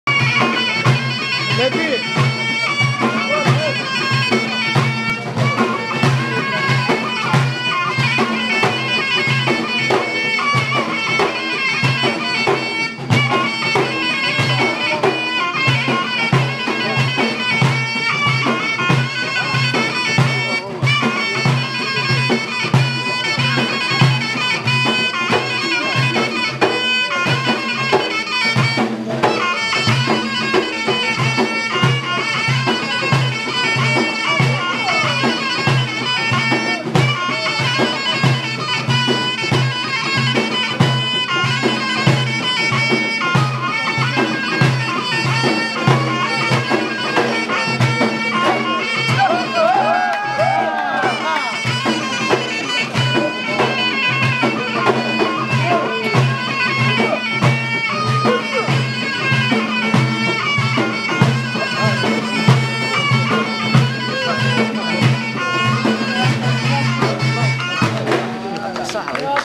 Bousaada
A traditionnal dance of Bousaada.